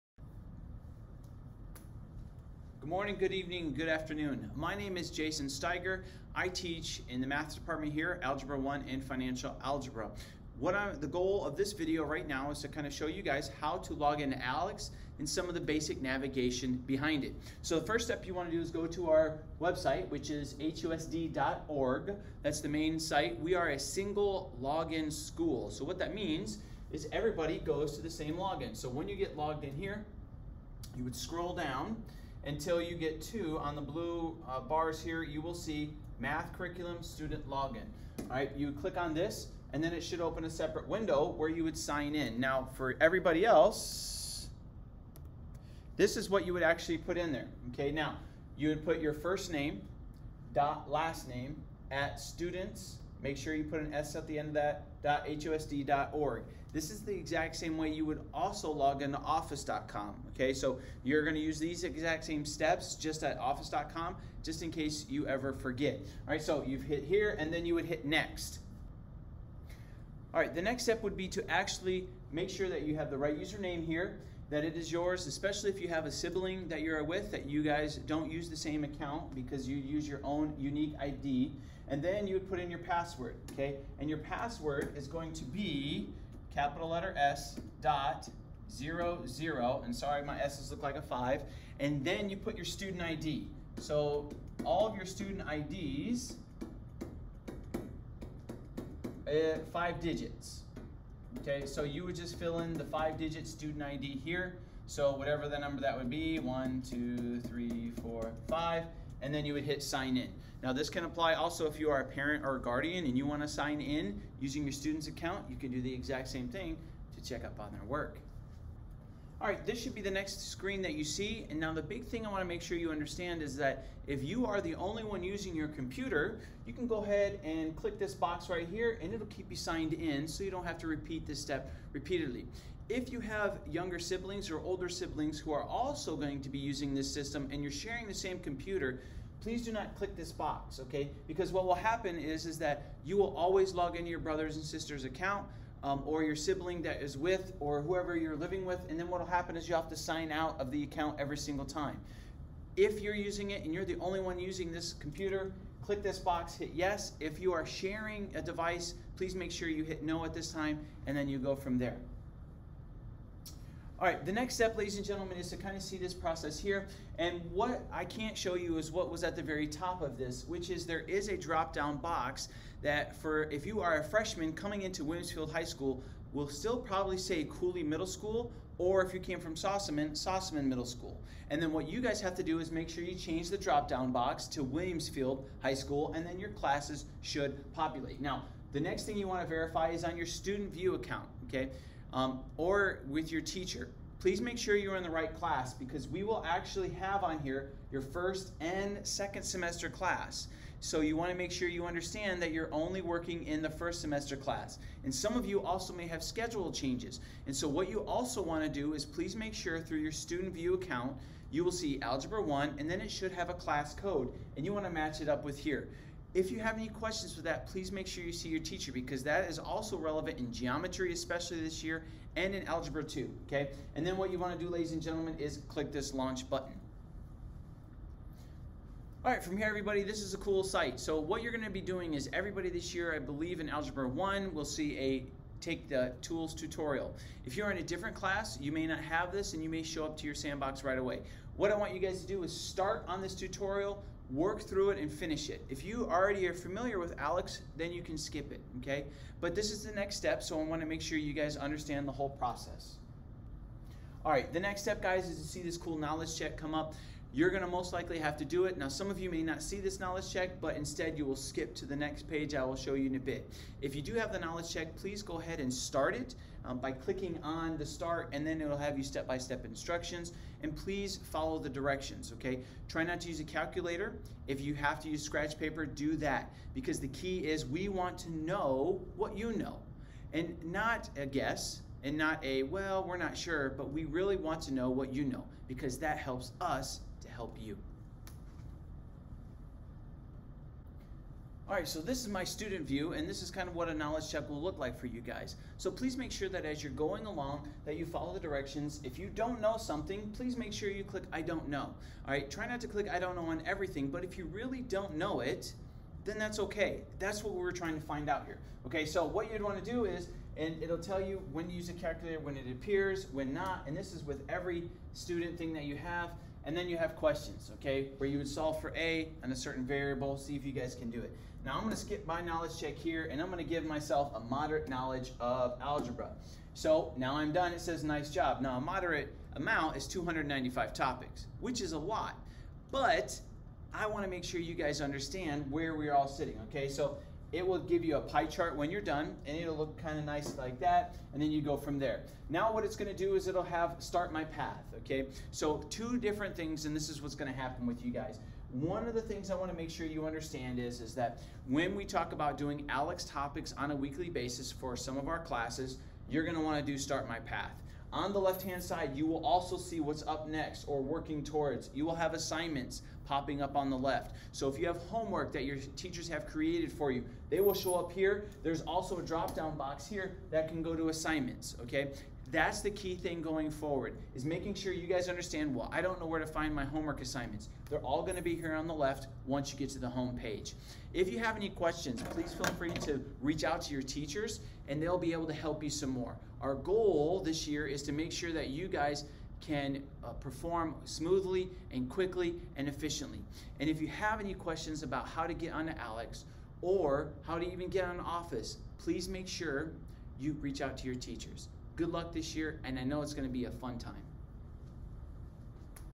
How to login to ALEKS
This step by step guide will help to facilitate any issues with Parents or Students trying to login to ALEKS.